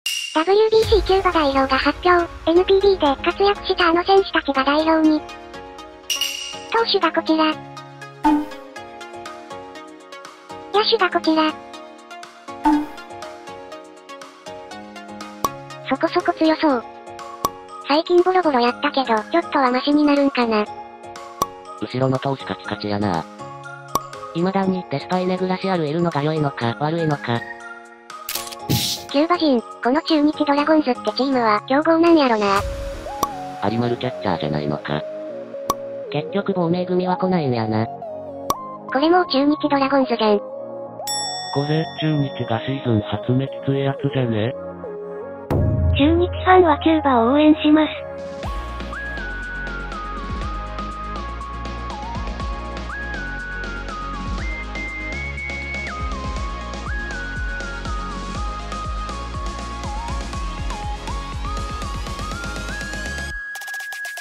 【必見】元ロッテの〇〇など、日本で活躍したあの選手たちも...「WBCキューバ代表発表！！」【なんJ】【ゆっくり解説】
プロ野球の時事ネタや歴史などのなんJの反応スレをまとめて、わかりやすく動画にしています。よろしくお願いいたします。